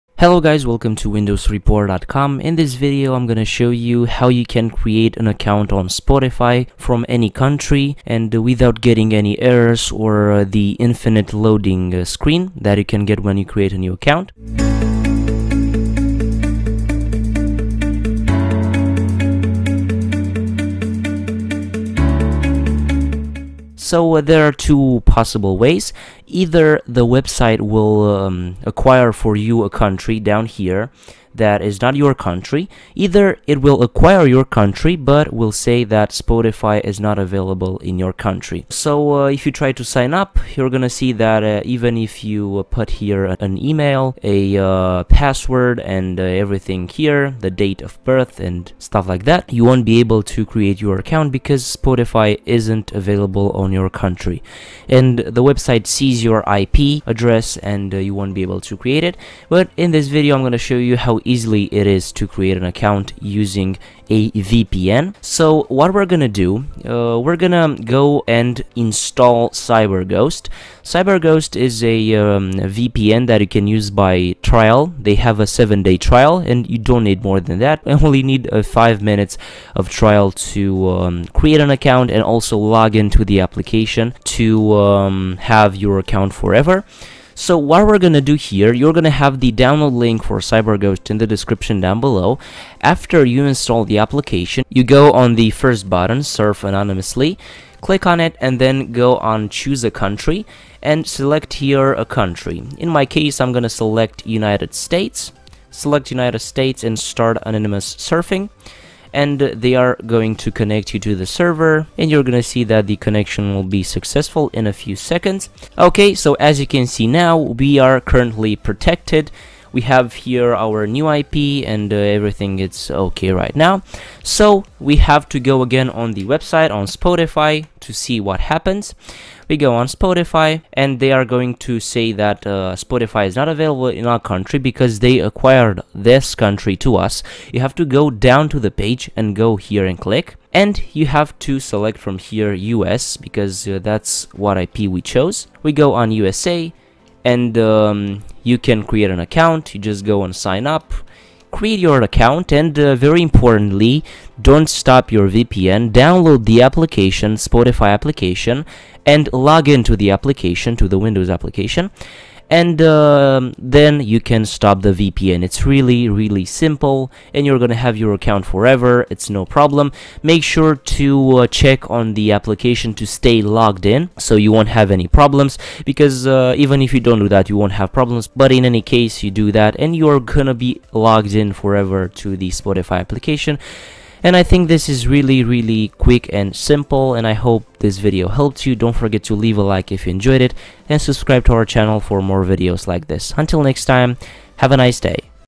Fix: Spotify can’t create Account (Loading Screen) - Create account from any country
| ⚙️Try Fortect to fix and optimize your PC like a Pro

In todays video, I'm going to show you how to create an account on Spotify from any country, very easily with a VPN called CyberGhost. Spotify site tracks your IP, but you can change it just to create the account and login to the application.